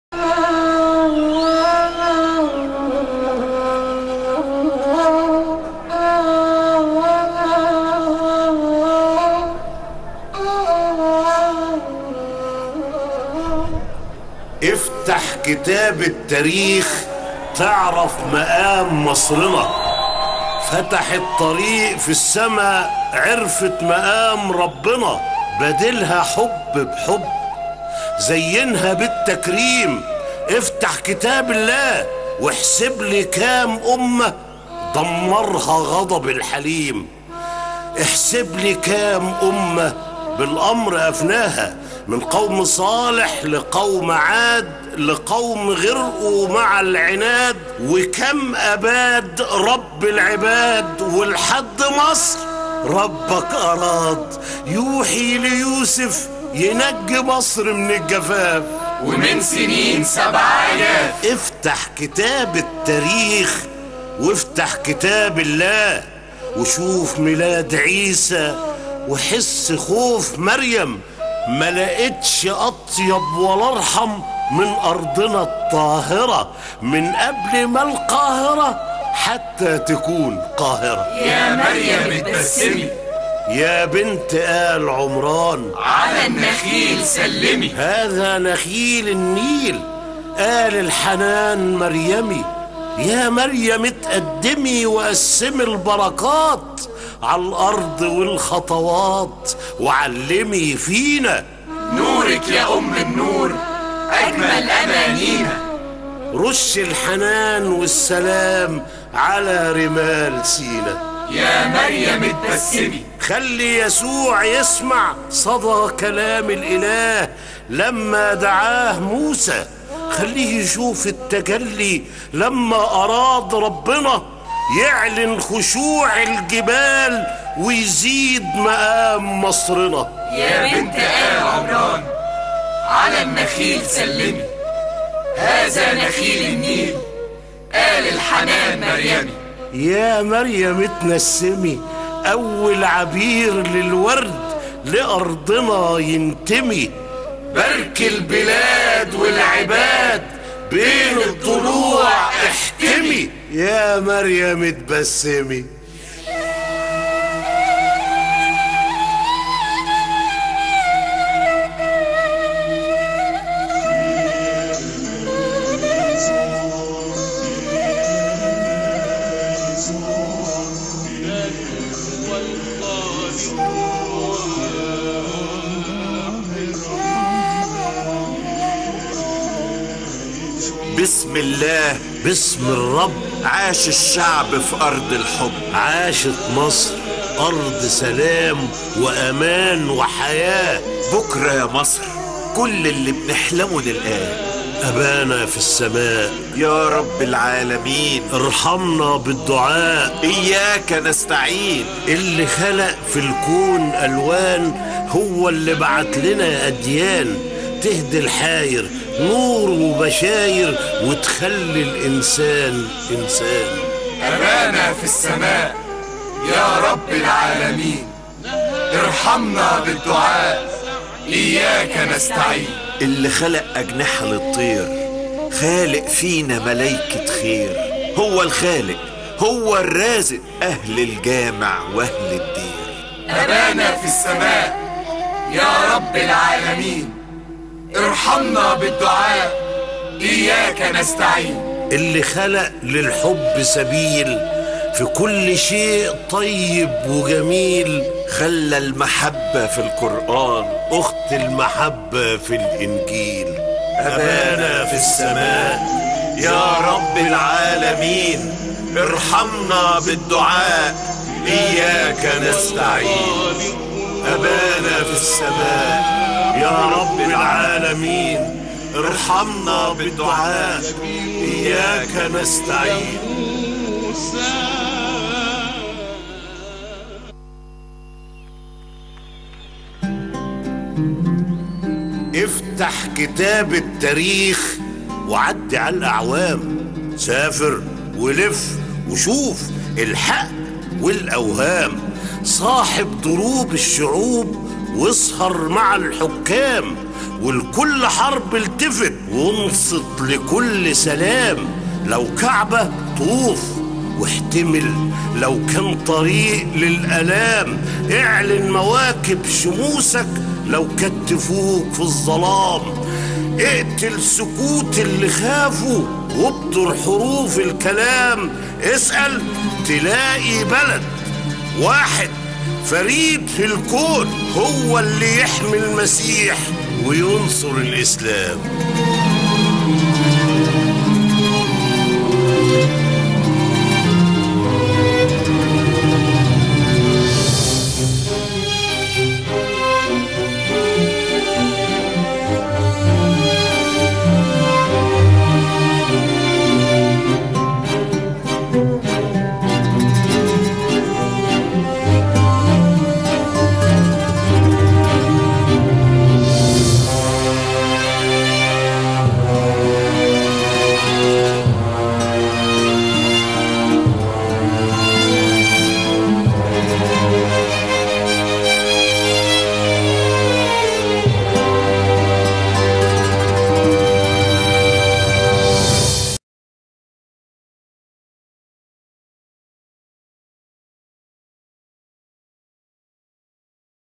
EGYPTIAN 40TH YEAR CELEBRATION FOR REGAINING SINAI IN YOM KIPPUR WAR
THIS IS AN ENGLISH CAPTION OF A VERY NICE POETIC CLIP BY SALAH ABDALAH AND HANY RAMZY WHICH DEALS WITH UNITY IN RELIGIONS. IT WAS PART OF THE FESTIVITIES HELD IN THE EGYPTIAN AIR FORCE STADIUM.